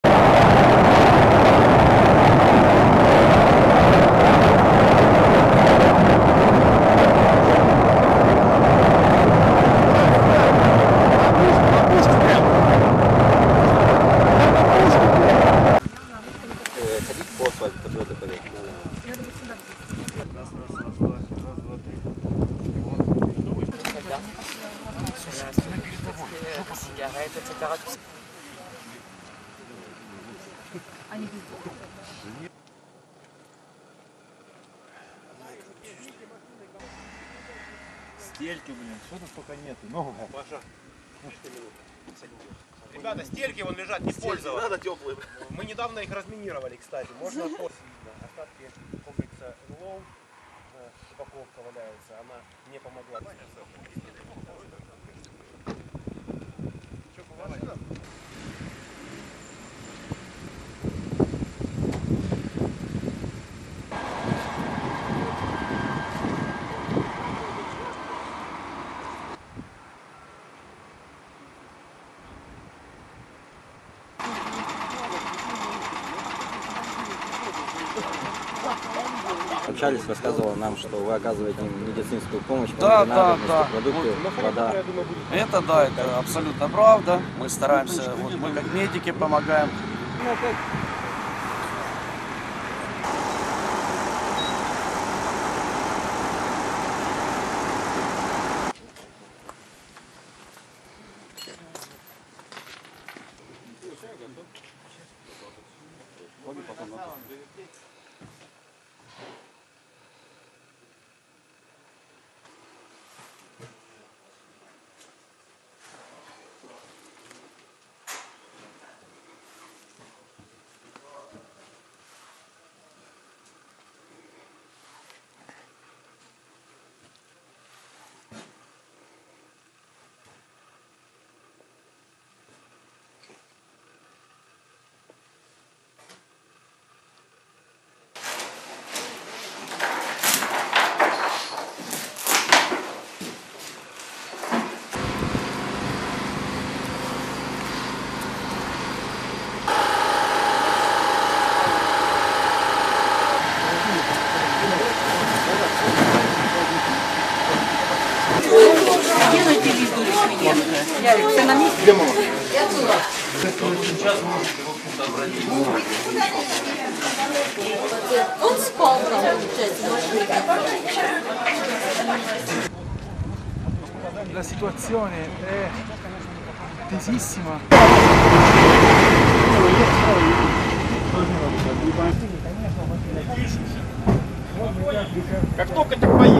Russian Troops Take Control Of Ukrainian Village Of Mykolaivka
Russian troops take control of Mykolaivka, a village in Ukraine.